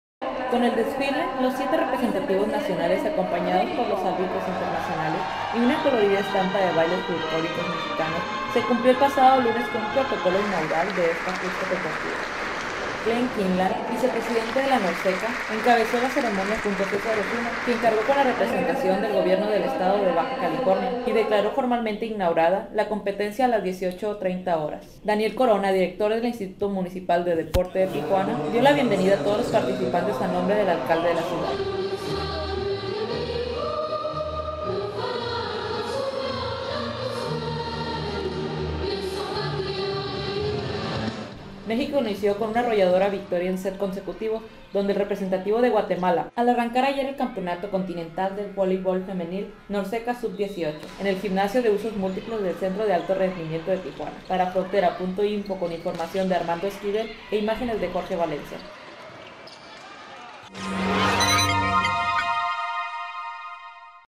Campeonato Continental de Voleibol Femenil
México inició con una arrolladora victoria en sets consecutivos sobre el representativo de Guatemala, al arrancar el pasado lunes el Campeonato Continental de Voleibol Femenil de Norceca Sub 18, en el gimnasio de usos múltiples del Centro de Alto Rendimiento de Tijuana.